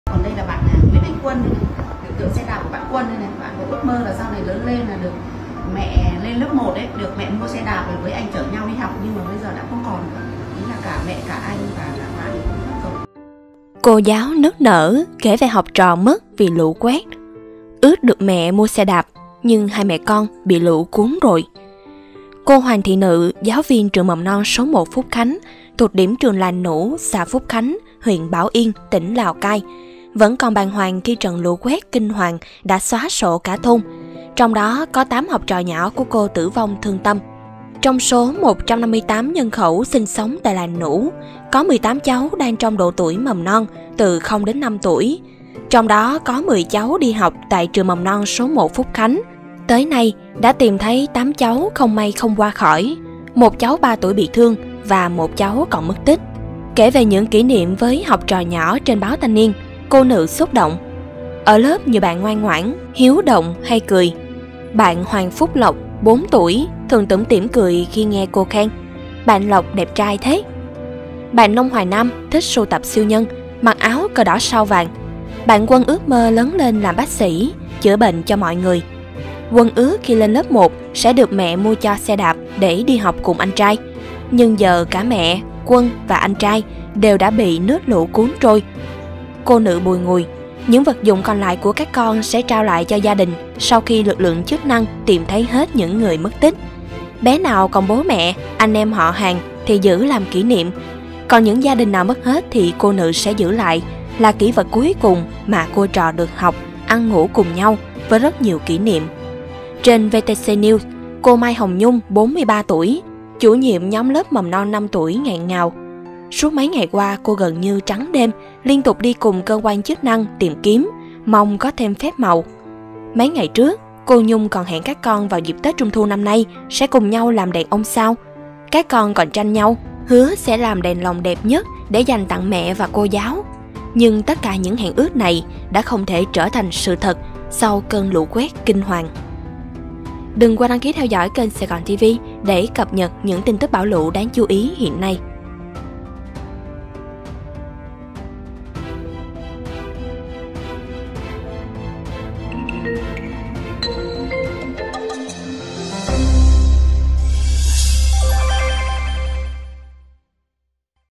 Cô giáo kể về 8/10 học trò mất vì lũ quét: Ước mẹ mua xe đạp, nhưng 2 mẹ con bị lũ cuốn | SaigonTV
Cô giáo kể về học trò mất vì lũ quét: Ước được mẹ mua xe đạp, nhưng 2 mẹ con bị lũ cuốn | SaigonTV

Bạn Quân ước mơ lớn lên làm bác sĩ, chữa bệnh cho mọi người. Quân ước khi lên lớp 1 sẽ được mẹ mua cho xe đạp để đi học cùng anh trai, nhưng giờ cả mẹ, Quân và anh trai đều đã bị nước lũ cuốn trôi.
| Theo Thanh Niên